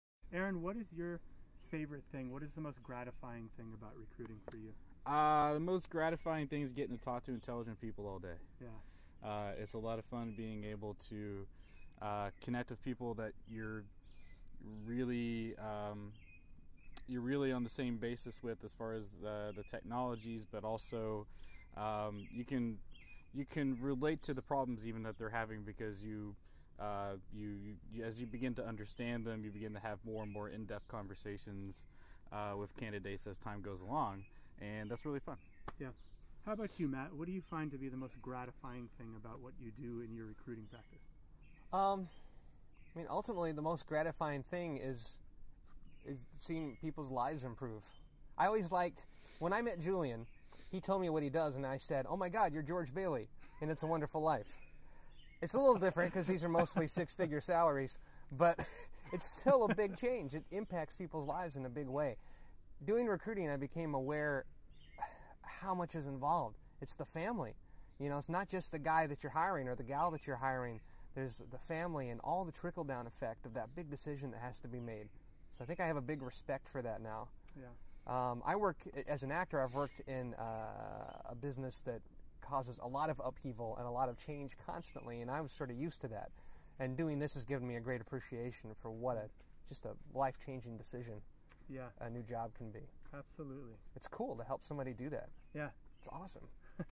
DyNexus Recruiting: What makes recruiting gratifying
Two DyNexus Recruiters talk about what they enjoy about the recruiting business.